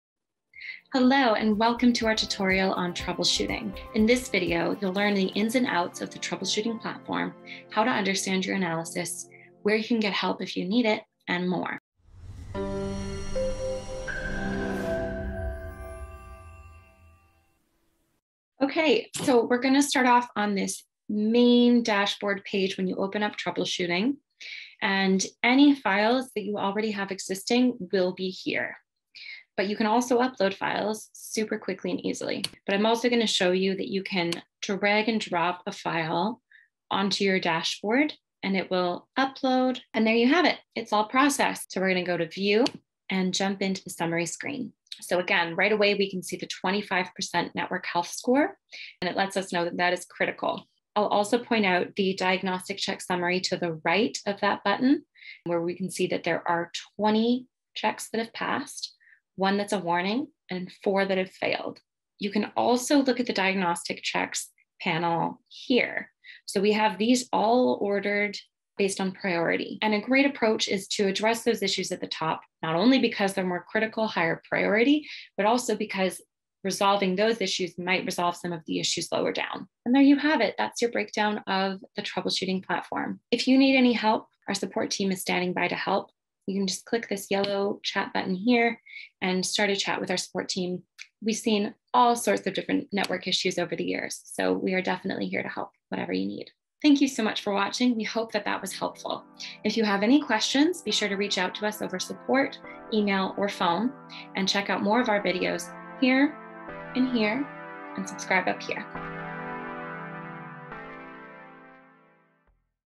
Visual BACnet Troubleshooting Tutorial
Visual BACnet Troubleshooting allows you to get an instant-read on Network Health, and pinpoint where there might be problems. This saves you time looking for what’s wrong, so you can focus your attention on fixing problems.

In this video, you’ll learn the ins and outs of the Troubleshooting platform, how to understand your analysis, where you can get help if you need it, and more.